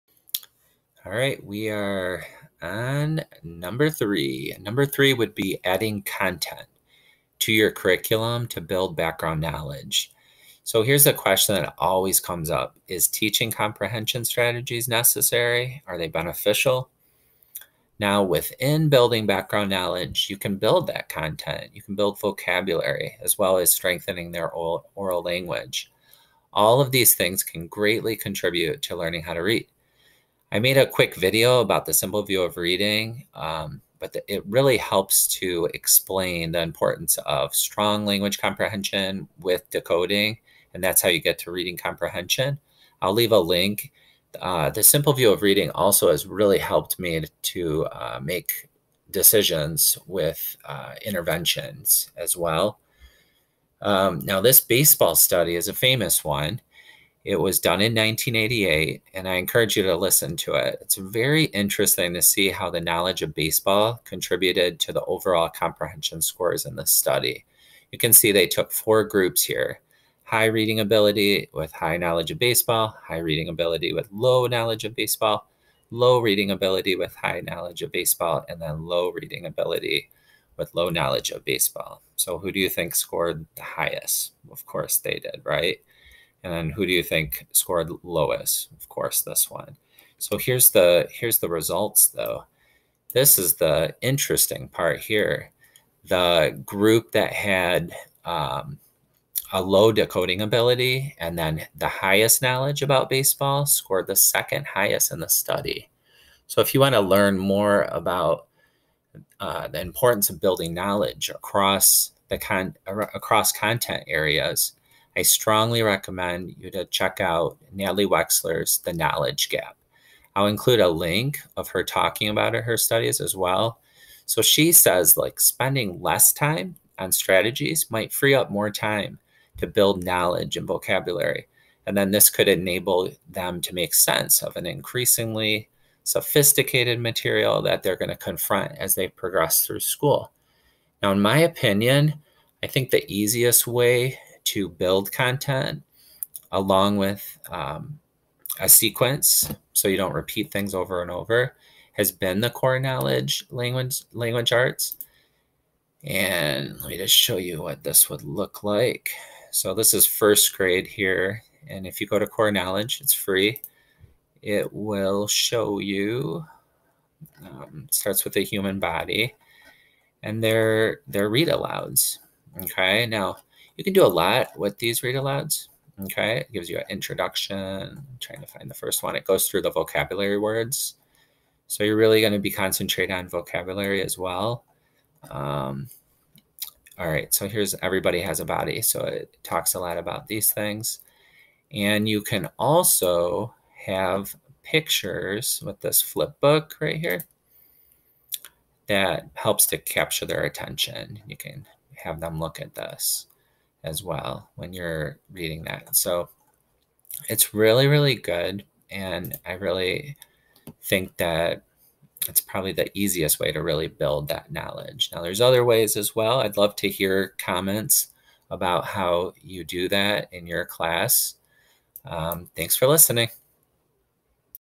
#3/10- Add Content to to Your Curriculum to Build Knowledge
#3/10- How to Add Science of Reading to Your Classroom

How can we improve reading comprehension? Have you ever taught how to find the main idea, author's purpose, sequence of events, or other comprehension strategies? How important it is to add content and build background knowledge to help students learn how to read? Is it better for them to discover this knowledge on their own, or should educators be telling them and teaching them what they need to know? How can they get the information they need to know to read and comprehend text?
Natalie Wexler's The Knowledge Gap blew my mind! If you haven't read it, I HIGHLY recommend it. If you are stretched for time, listen to the a 1 hour webinar. If you have any suggestions to build background in content areas, please share.

*Linda Farrell (One of my favorite reading experts) discusses what the Simple View of Reading is and why it is so important:

*Watch: Robert Pondiscio, Director of Communications at Core Knowledge®, speak about the fundamentals behind the Common Core State Standards.